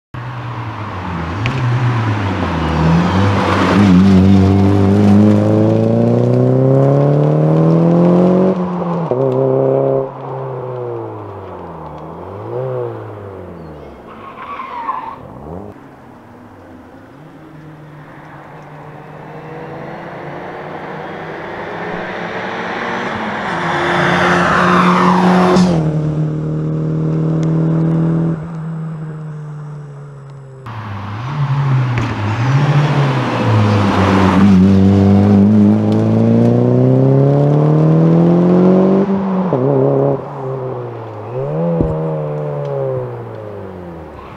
BuBi Rallye 20' - #26 mit U.Latz / J.Kemner im Mazda 323 GT-R
Hier seht ihr eine zusammenfassung vom Team U.Latz / J.Kemner im Mazda 323 GT-R .

Die "Buten un' Binnen" Rallye ist eine jährliche Motorsportveranstaltung in Norddeutschland.
Sie bietet abwechslungsreiche Strecken und verschiedene Kategorien für Teilnehmer.
Zuschauer können die spannenden Rallyewagen entlang der Strecke erleben.
Es ist eine beliebte Veranstaltung, die den Rallyesport fördert und Fahrer sowie Fans zusammenbringt.